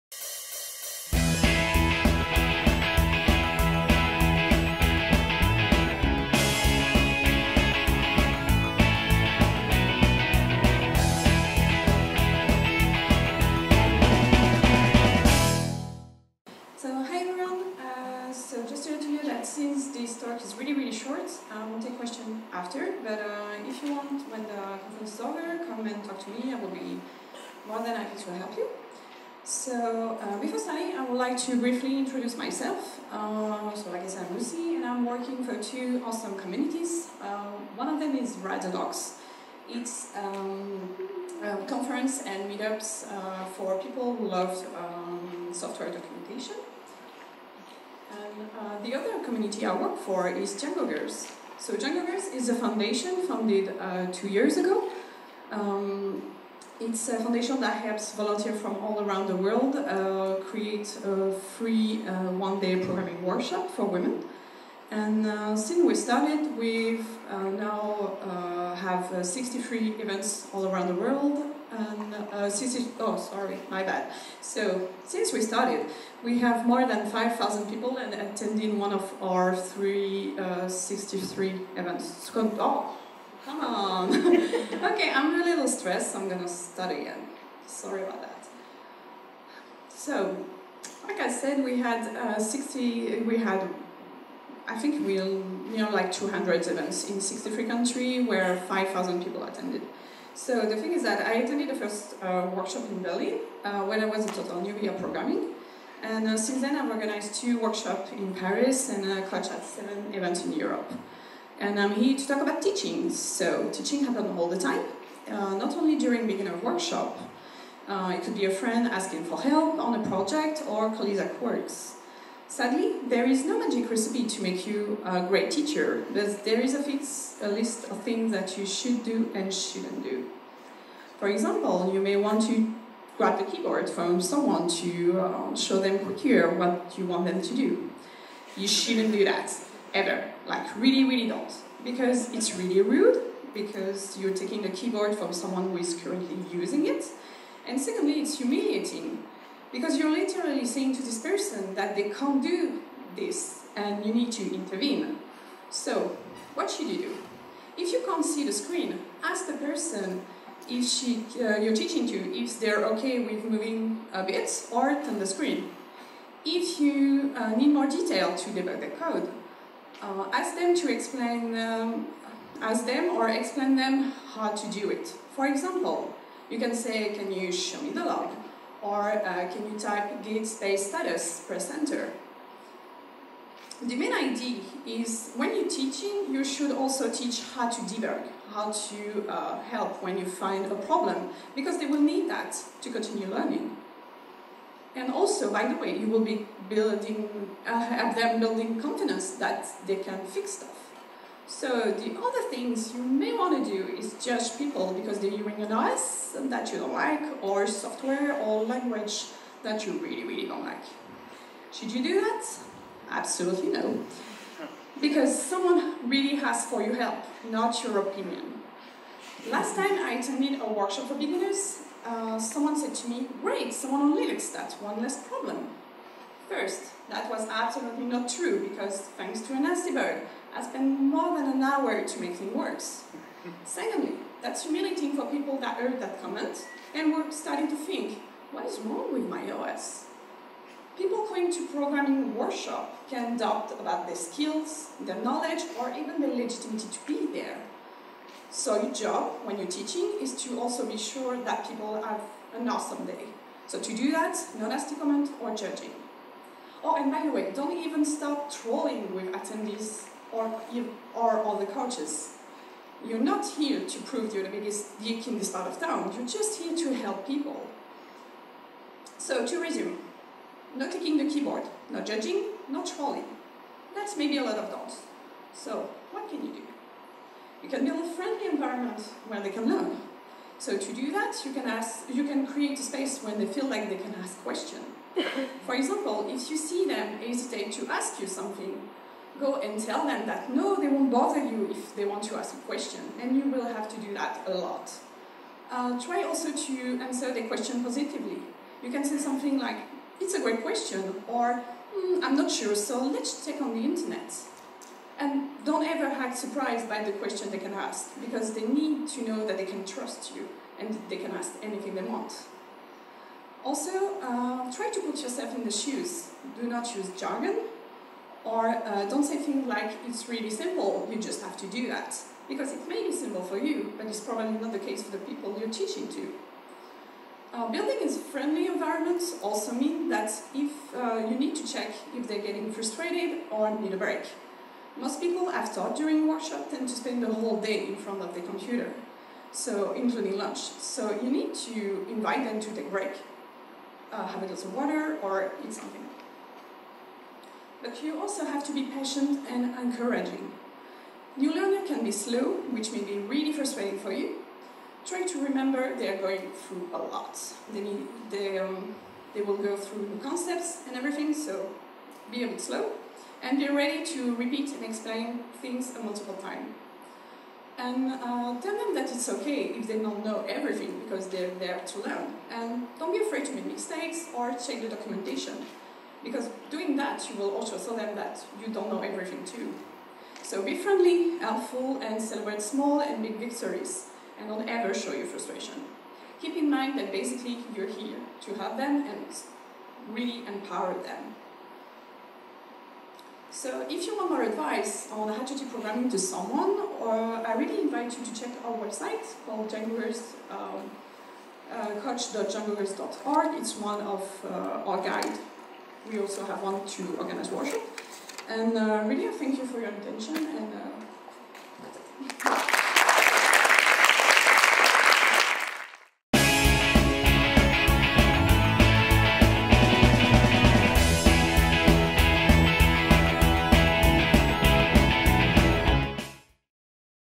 AlterConf Paris 2016 - Teaching programming to someone: do’s and don’ts by Lucie Daeye
Teaching programming to someone: do’s and don’ts by Lucie Daeye

"Don't ever touch the keyboard" is one of the main rules of teaching programming to someone. But what else should you know before mentoring someone? This short talk will focus on what you can do to help and empower new learners.